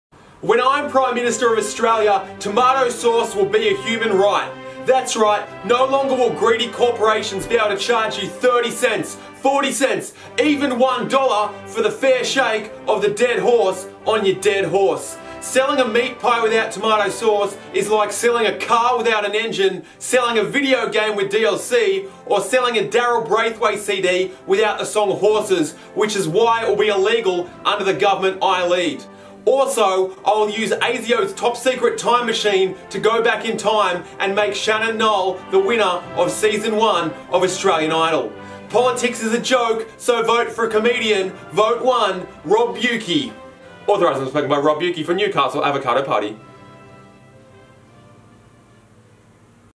WHEN I'M PRIME MINISTER EP 2: TOMATO SAUCE
When I'm Prime Minister of Australia tomato sauce will be a human right!